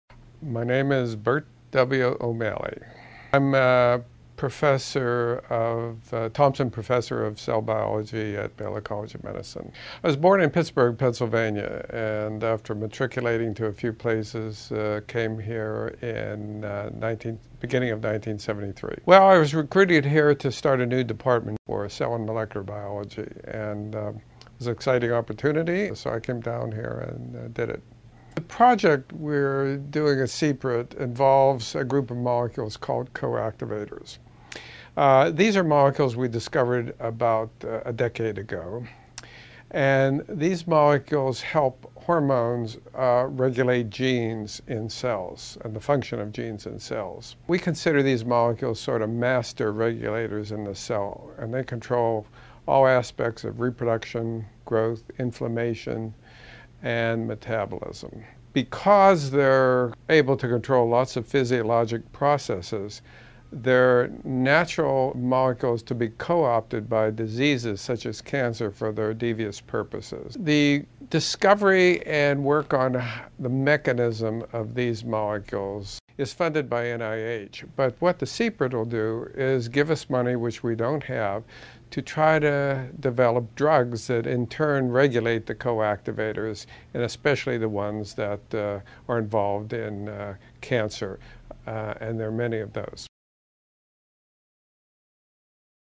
Dr. Bert O'Malley
Dr. Bert O'Malley is chair and professor of molecular and cellular biology at Baylor College of Medicine. His primary research focus is on hormone action and gene expression: coactivators and corepressors.